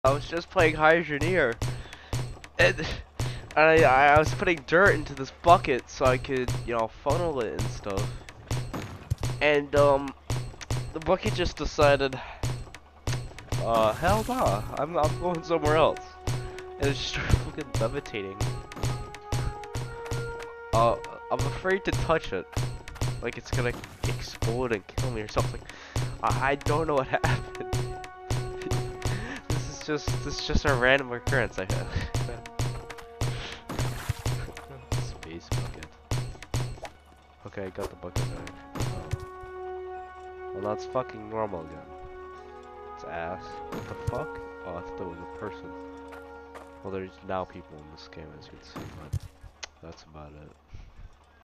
flying bucket
I was just playing and my bucket starting flying